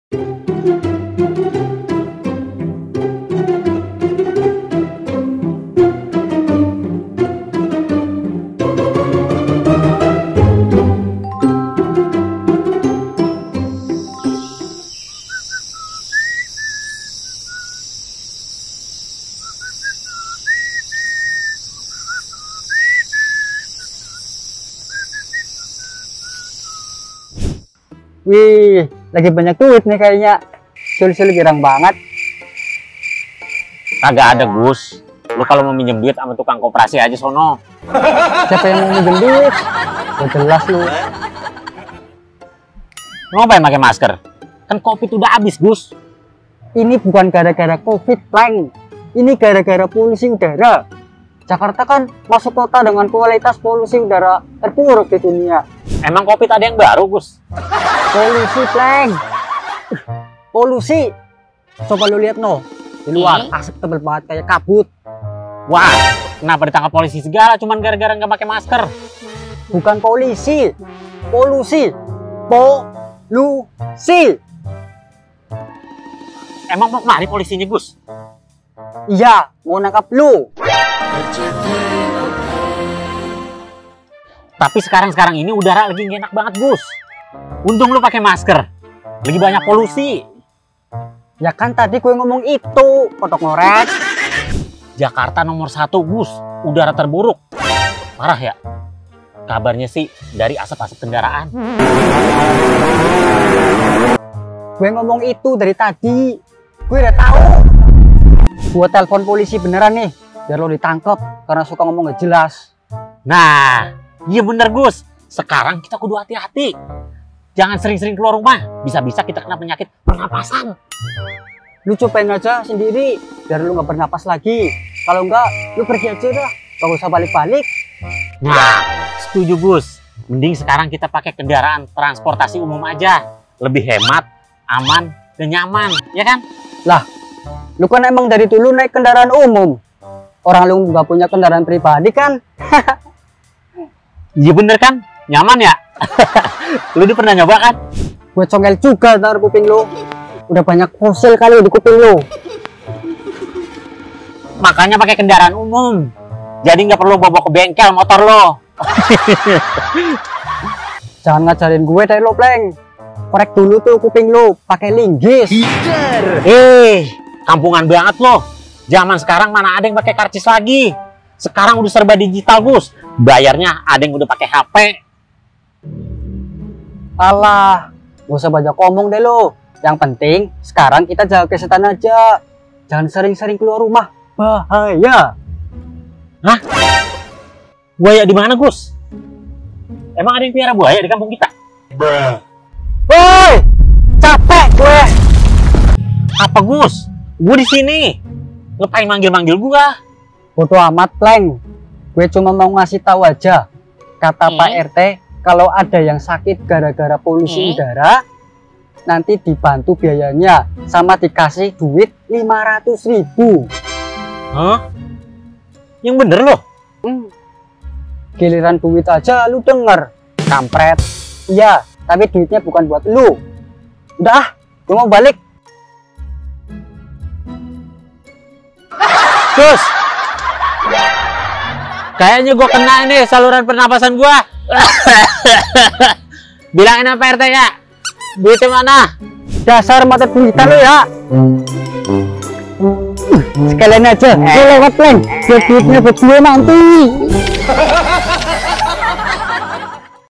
DEMI UANG SANTUNAN, RELA PURA - PURA SAKIT | KENA SERAGAN POLUSI UDARA
Pada suatu sore di kota kecil, warga desa terbangun oleh kabar buruk. Polusi udara yang parah telah melanda kota mereka. Asap hitam menutupi langit, membuat matahari terlihat seperti lampu neon yang redup. Dua orang pemuda berdialog di taman kota untuk membahas apa yang harus dilakukan.

Ditonton yukk videonya, Semoga dapat menghibur...